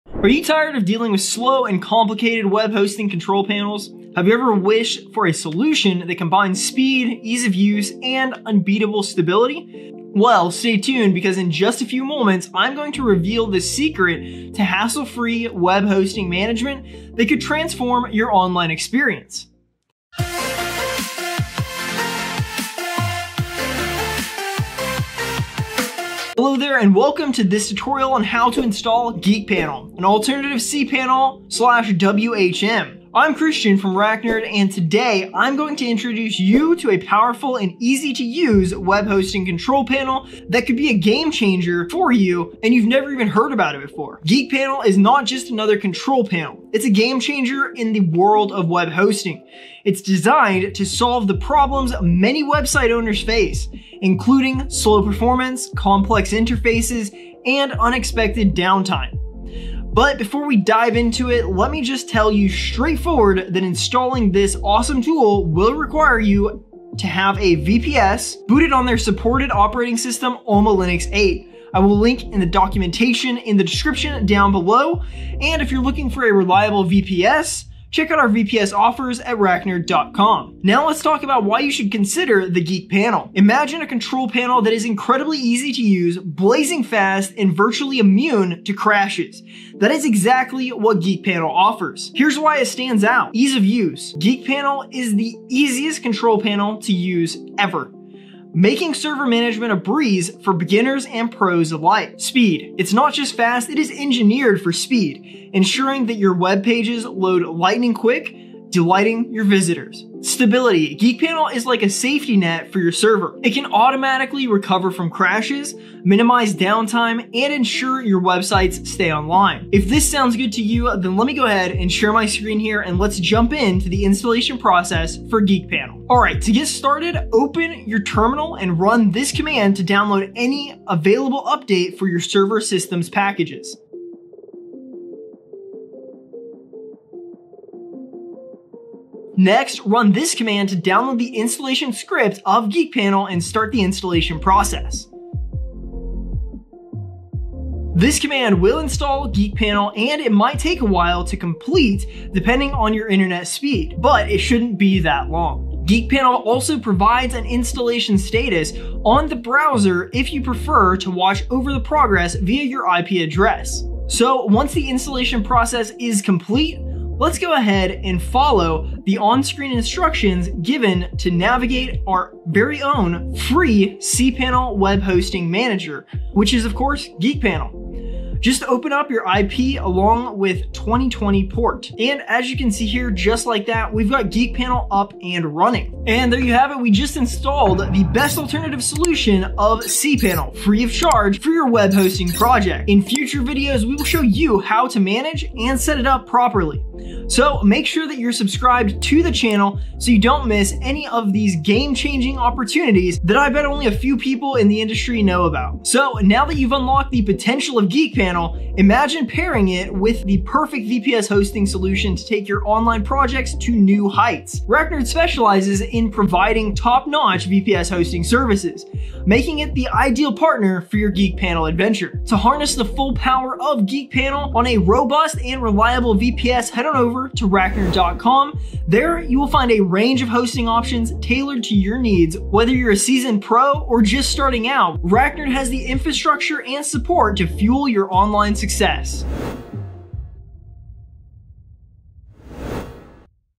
How to Install Geek Panel (Alternative to cPanel/WHM or BETTER?!)
Welcome to our video on How to Install Geek Panel (Alternative to cPanel/WHM or BETTER?!).

Are you in search of a powerful and feature-rich alternative to cPanel and WHM for managing your web hosting environment? Look no further! In this comprehensive tutorial, we'll show you how to install Geek Panel, a robust control panel that could be just what you need.

Geek Panel offers a user-friendly interface with a wide range of tools for managing your websites, domains, databases, and more. Whether you're a seasoned webmaster or a newbie, you'll find this guide helpful.

⏳ Timestamps:
00:00 Intro
00:26 Overview
02:06 Tutorial
03:05 Next Steps

### Linux Commands ###
dnf update -y
curl -s -o latest -L && chmod +x latest && ./latest
### Linux Commands ###

Moving forward...

☐ Liked?
☐ Shared?